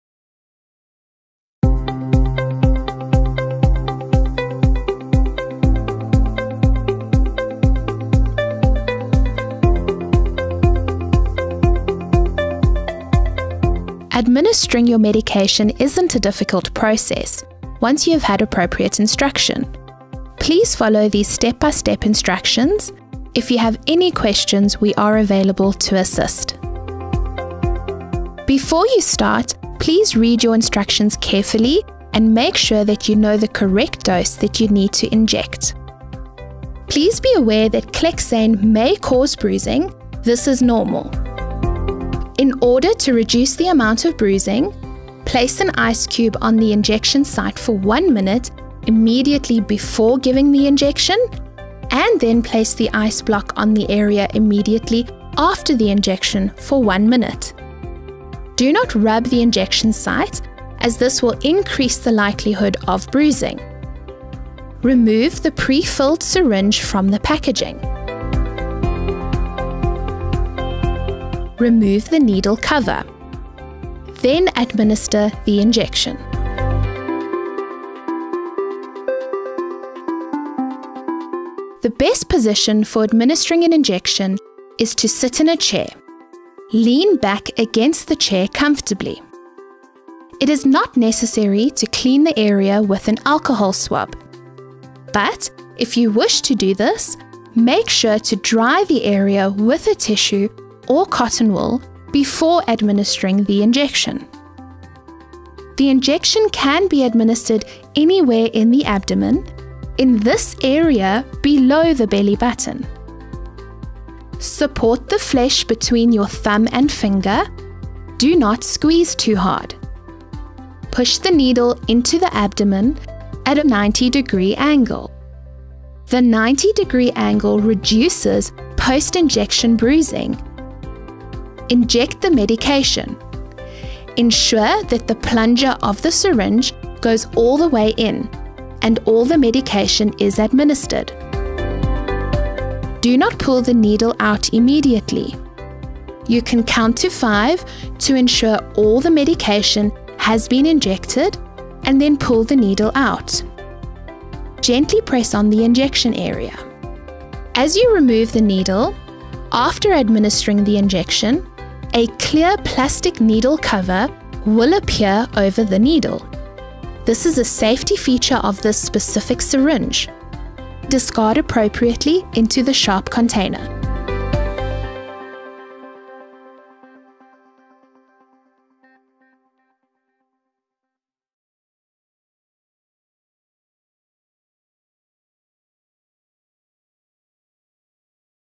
Clexane - How to use!
This video tells you HOW TO USE the Clexane 40mg injection, which is a pre-filled syringe administered subcutaneously ideally in the abdomen area beneath the belly button.

We will cover all the important questions and instructions like what is Clexane 40mg, how to use it, how to inject it, how to store it, where to inject it, when to take it and much more! This is a full tutorial on the Clexane 40mg injection.

Clexane is a blood thinner medication containing Enoxaparin. Clexane has been known to cause bruising (common side effect) around the injection site and this is normal, we do share a few tips on how to reduce the bruising.

Chapters:
00:00 Introduction
00:09 Read your instructions
00:38 Bruising
00:42 Step 1
01:04 Step 2
01:18 Step 3
01:54 Step 4
02:01 Step 5
02:12 Step 6
02:37 Needle Cover
02:50 Step 7
02:55 END

Let us know what other medications we should do "HOW TO" videos on for you?

Always remember with Vitalab: Arrive as a Patient, Leave as a Parent.